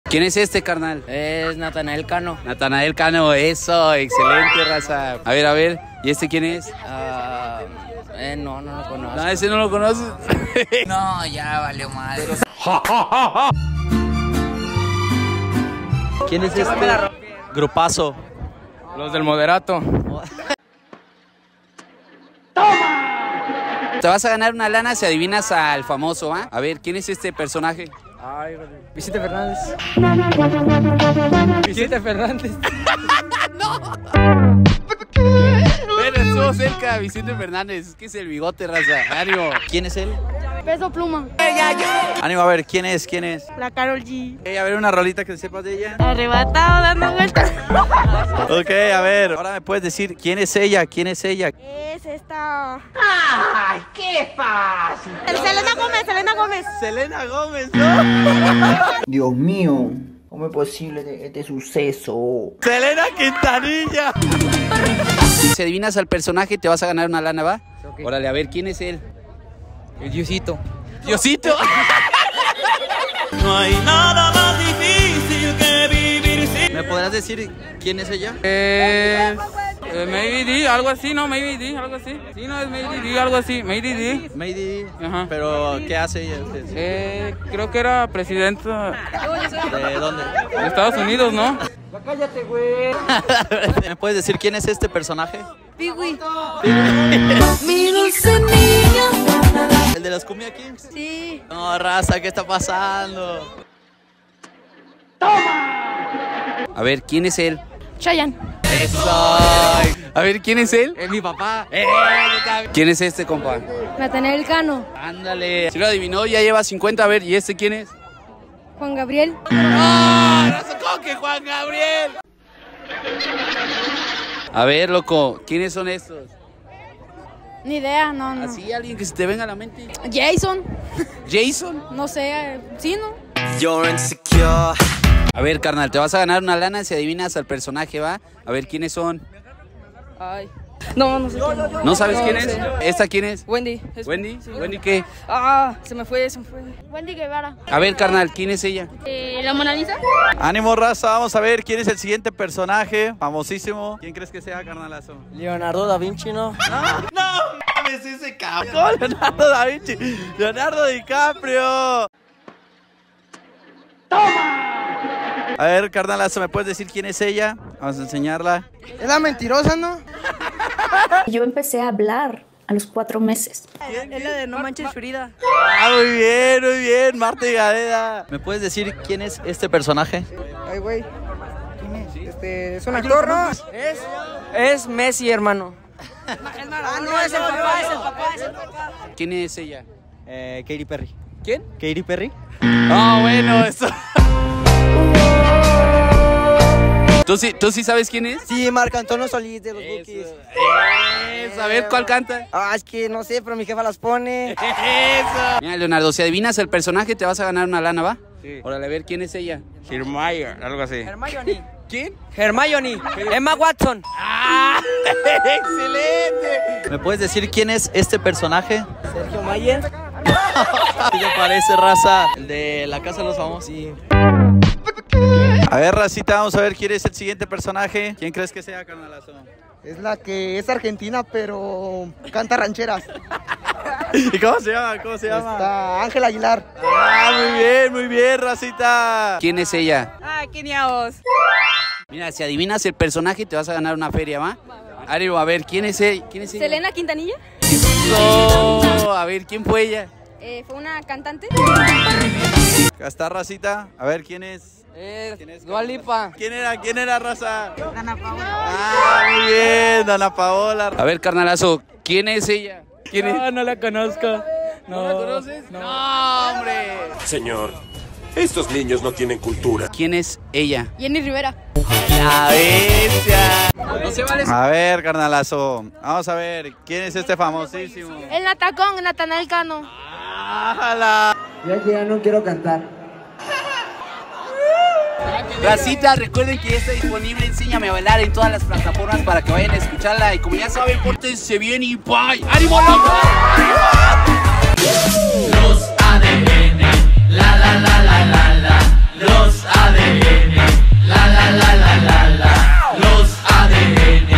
Los niños no conocen a estos FAMOSOS 😂 | Los ADN
Salimos a preguntar si conocían a estos famosos y aqui esta el resultado XD

EDICIÓN: @AbrahamSantoyo